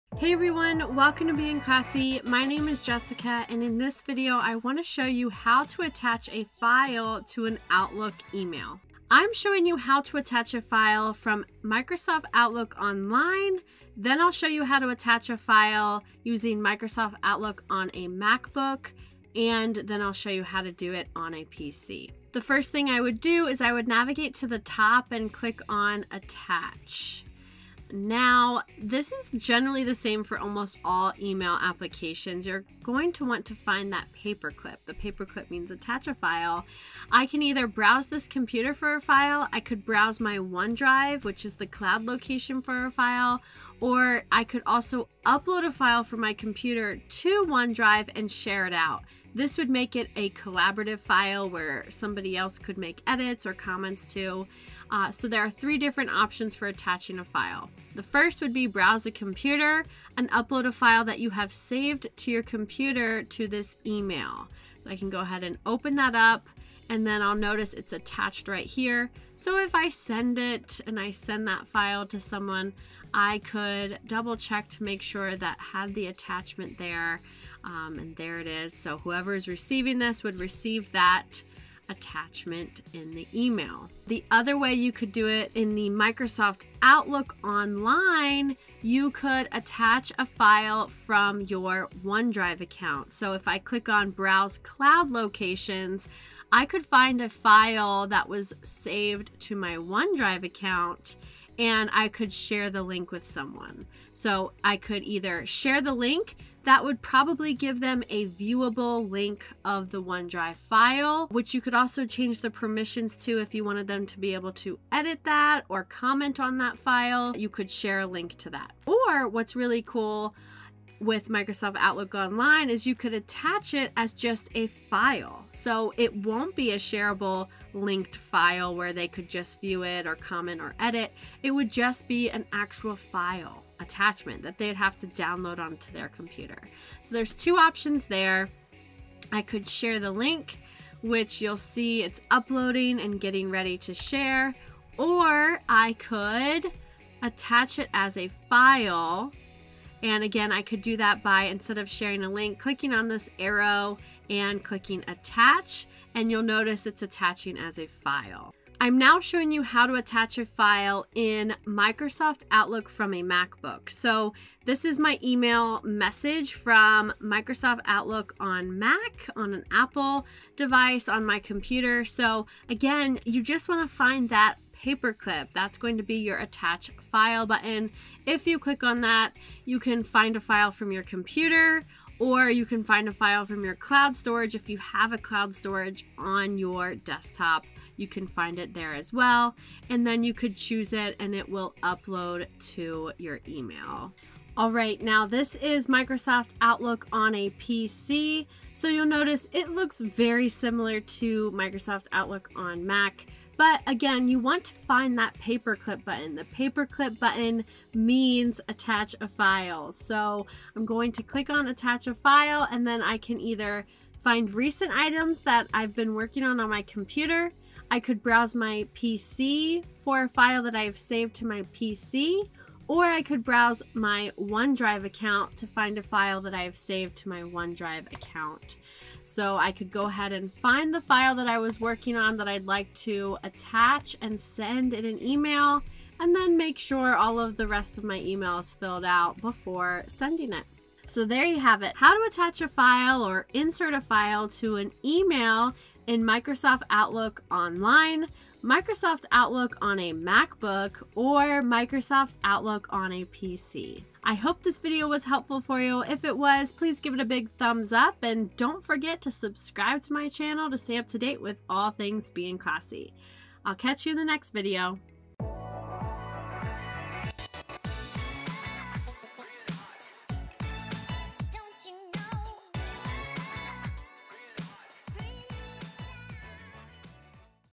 How to Attach a File to an Email in Outlook
In this video, I show you how to attach a file to an email in Microsoft Outlook.  Whether you are new to Outlook or just looking for simple tips, I truly hope you find this video useful. Thank you!

Facebook: @beinclassy Want to show your support and gratitude for this video?